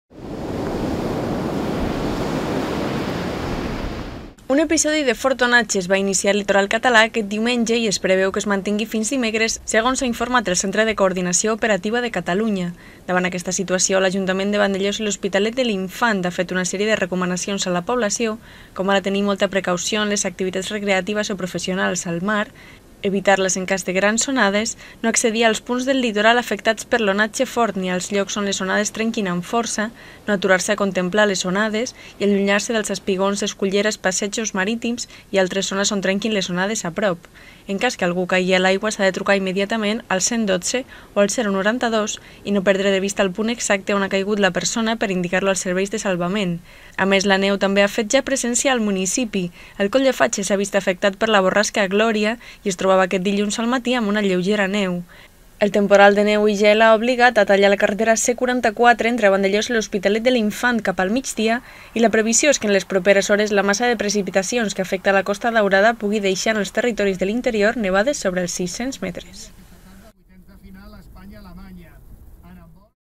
Fort temporal al municipi
Fort onatge i neu al nostre municipi amb motiu de la borrasca Glòria. L'Ajuntament ha comunicat una sèrie de recomanacions a la població per tal d'evitar riscos innecessaris.